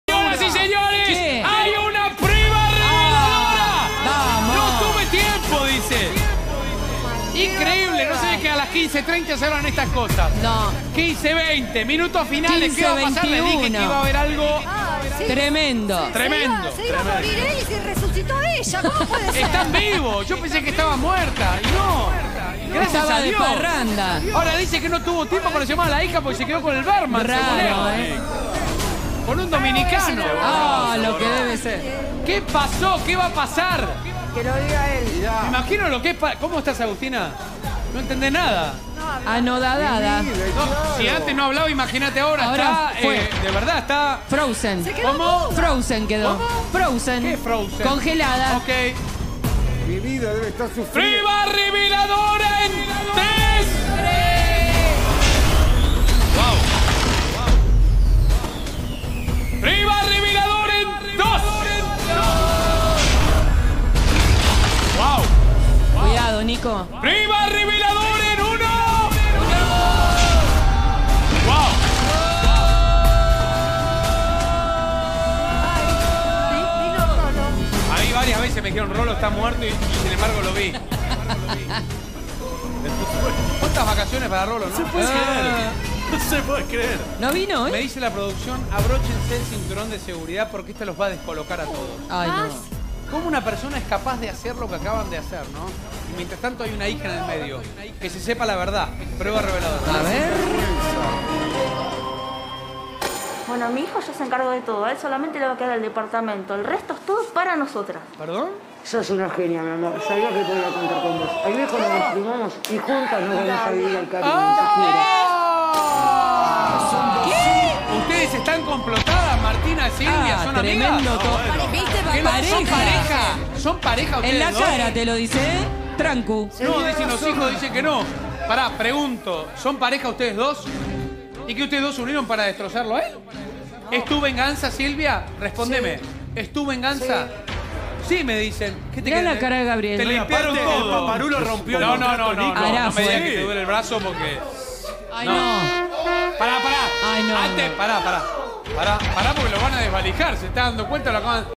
Martina y Silvia tramaron el engaño a Gabriel
En nuestra primera prueba reveladora descubrimos que Martina y Silvia son pareja y ambas se unieron para sacarle todo a Gabriel.